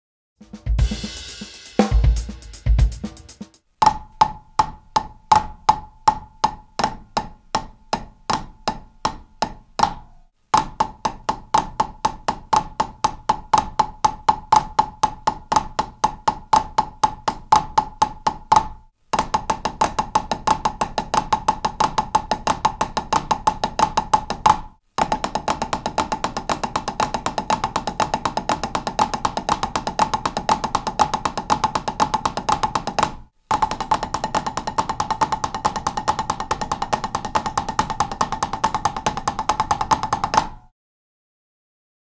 Back to Basics: 2. Flam Single Stroke Roll / Left Lead
Sweet flam exercise from Stick Control.